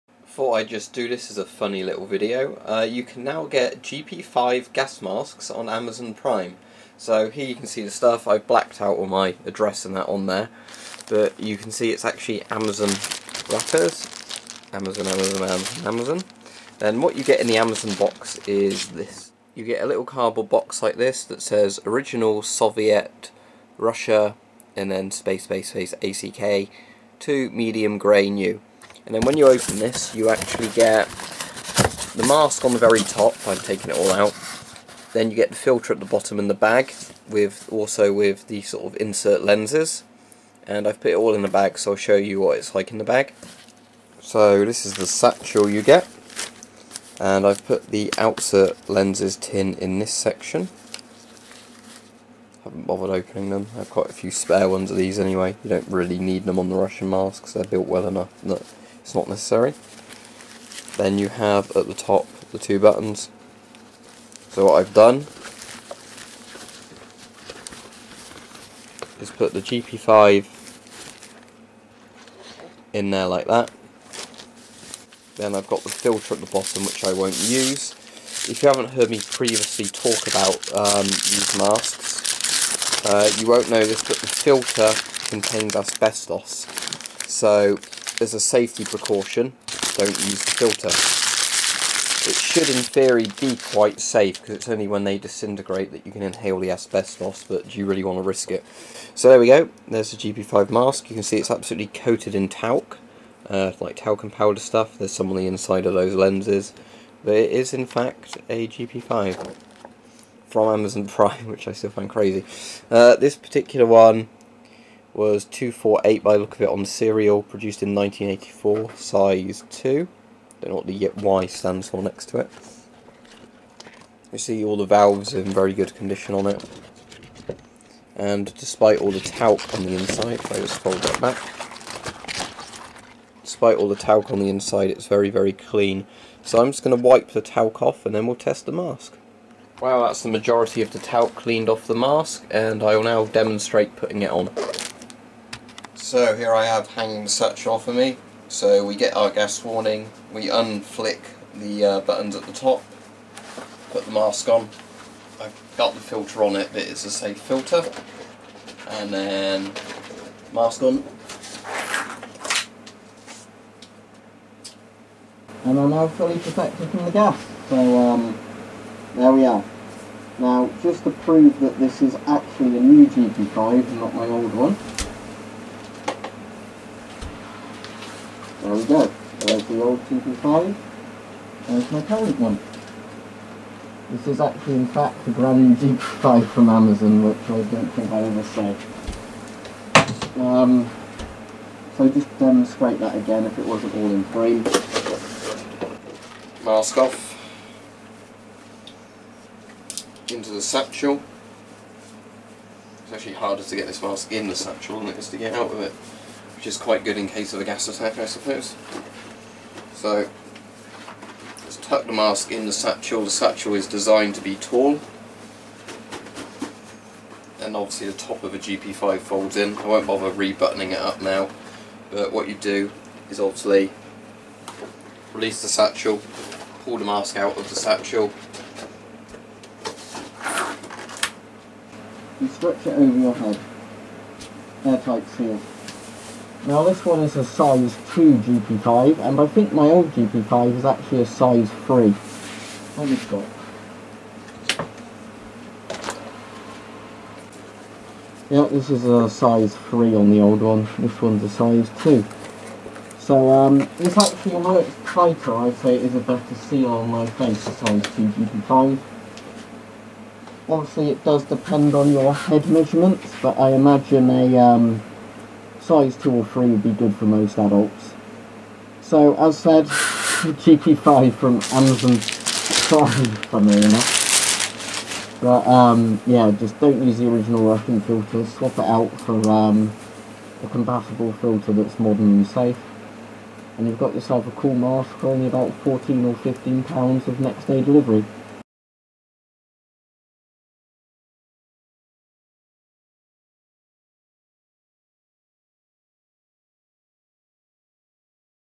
Soviet GP-5 Gas Mask on Amazon Prime
You can now buy Soviet GP-5 Gas masks directly through Amazon Prime and have them delivered the very next day.
I ordered one out of curiosity, the mask came with a bag, filter, and anti-fog inserts in a cardboard box, once opened you can store everything inside the bag.
The mask is very clean and still coated in its original talcum powder.
This was a Size 2 mask and cost around £14, the price seems to vary depending on mask size and stock.